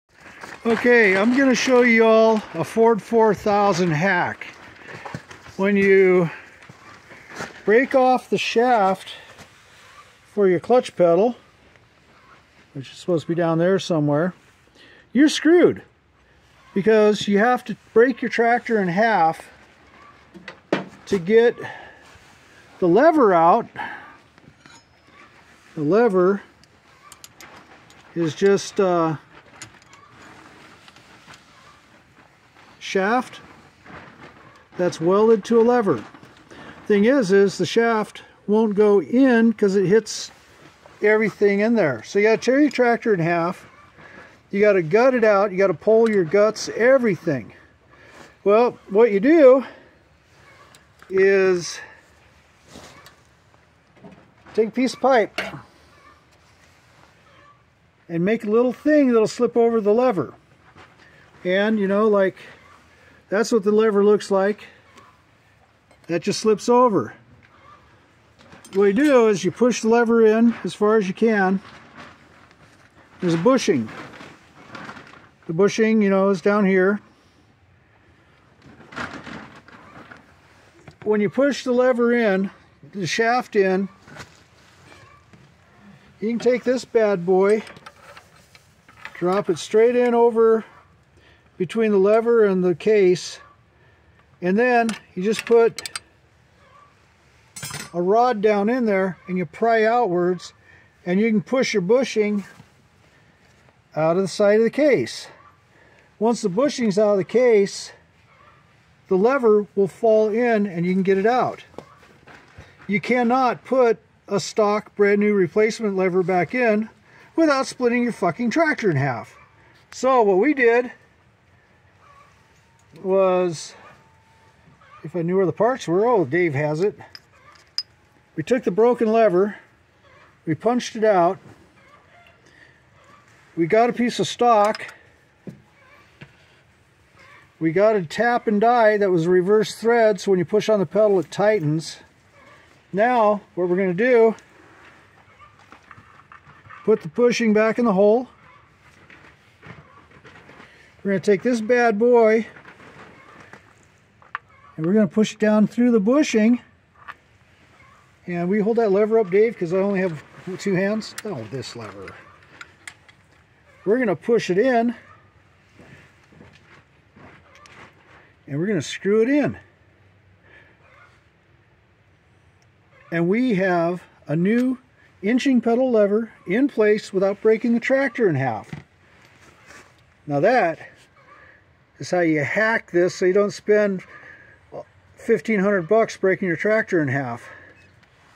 Ford 4000 Hack
How to replace a broken creeping clutch shaft without breaking the tractor in half.